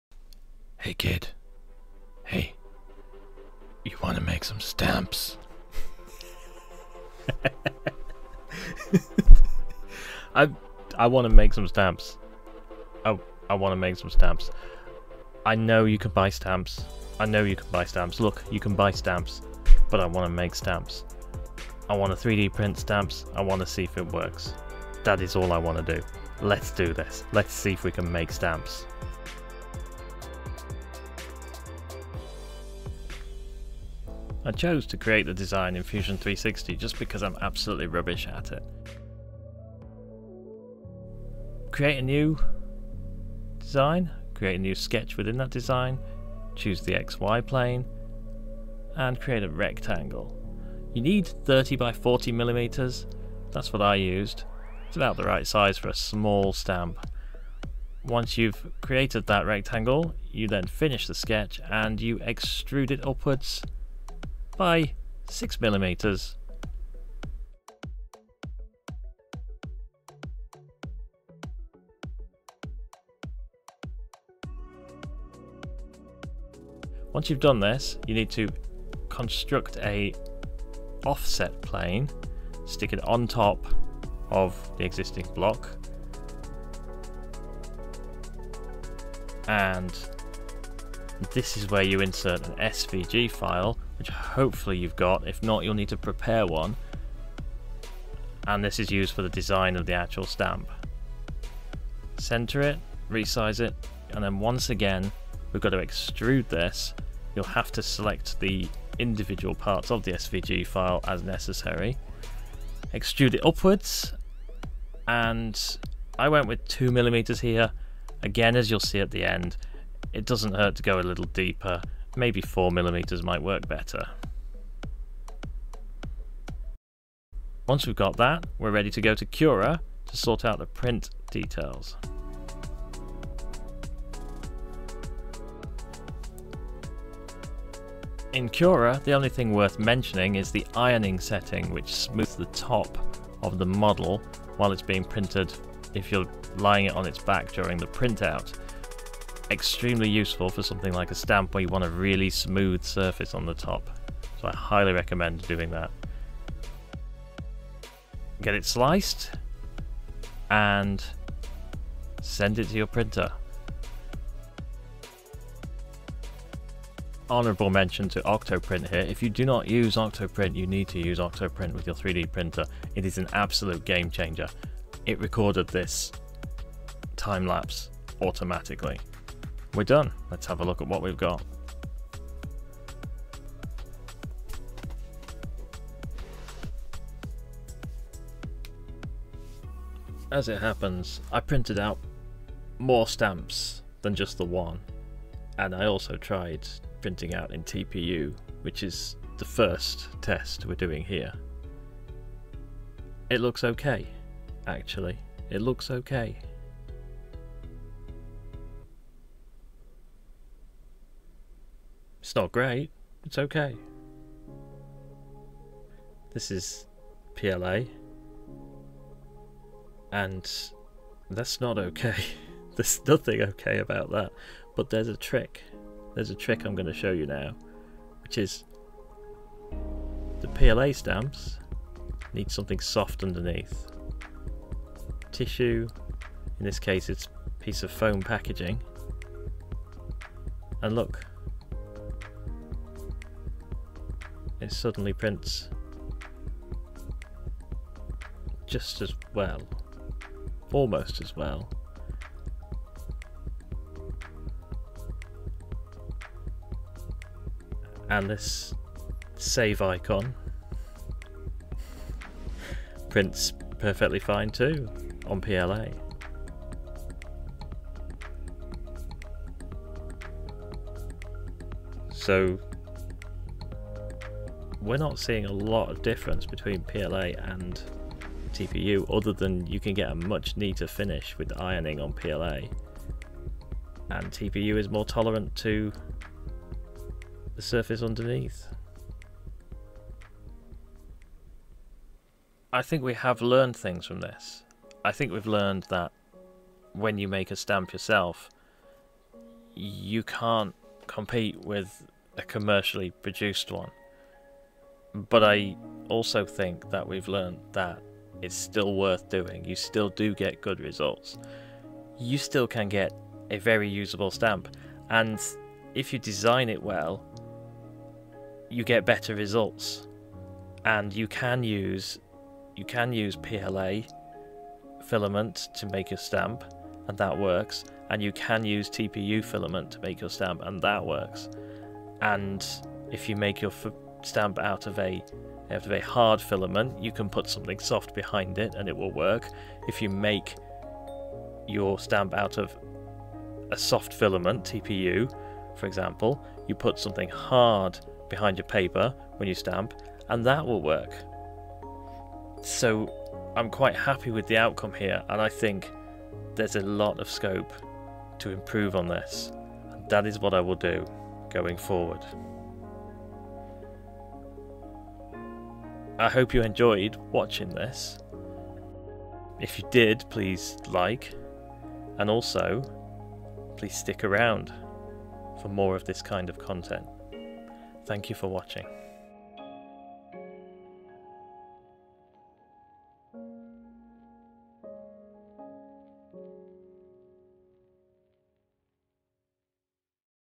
Can You Make 3D Printed Stamps? I Tried...
I want stamps. I'm too lazy and cheap to design a stamp and have it printed commercially.
Just kidding, of course. I've done this in the past and it actually works OK, but there's still the unanswered question: Can you make stamps on a 3D printer?
Unanswered until now...join me in a laid back exploration of how to make stamps, from start to finish.